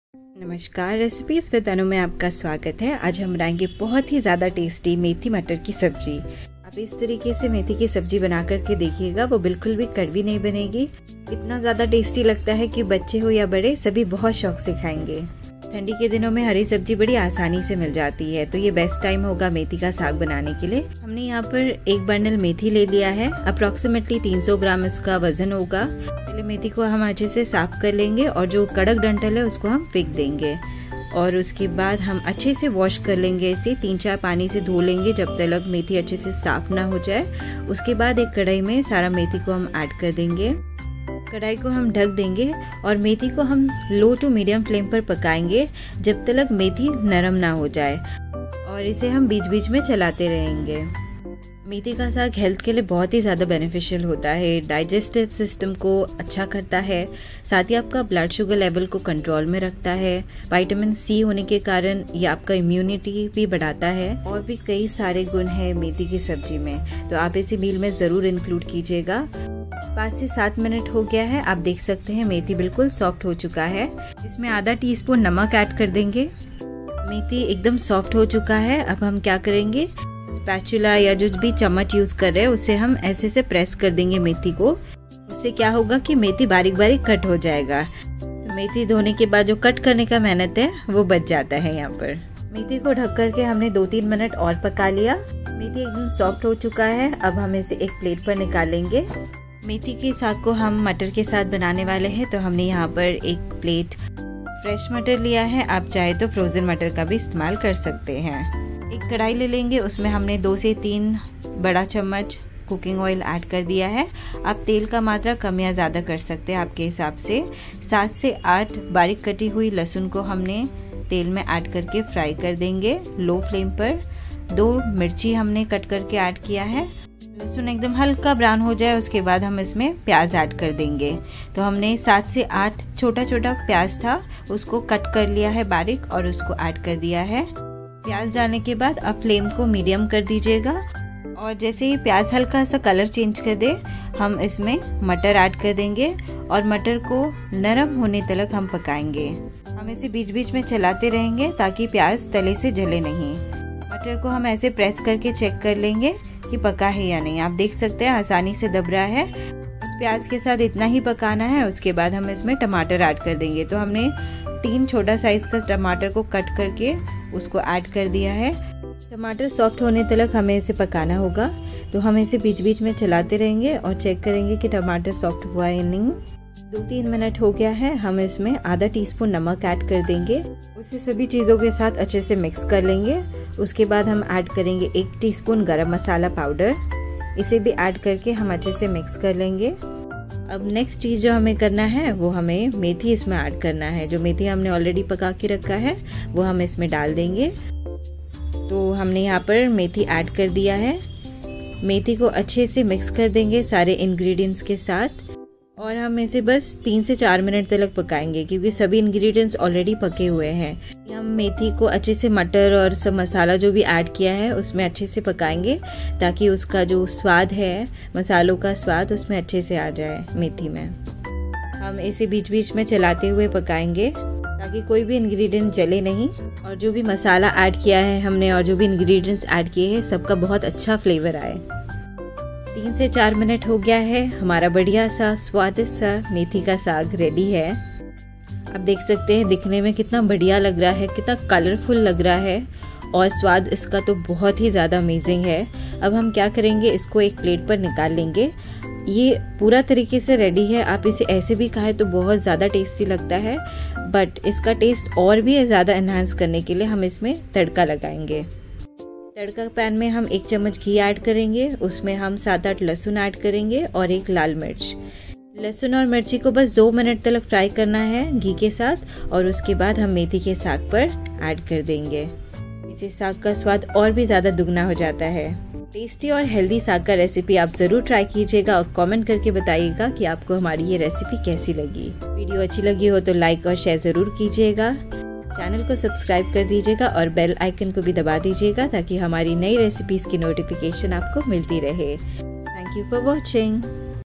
मेथी मटर की सब्जी ऐसे बनाये कि सब उंगलियां चाटते रह जाए । Methi Matar ki sabji। Methi ka Saag recipe
Ingredients List-
Methi leaves
1 tsp salt or as per taste
2-3 tbsp oil or as per preference
7-8 garlic
6-7 small chopped onion
2 red chilies
Green peas
3 small size tomatoes chopped
1 tsp garam masala

Tadka:
1 spoon ghee
1 red chilli
5-6 garlic cloves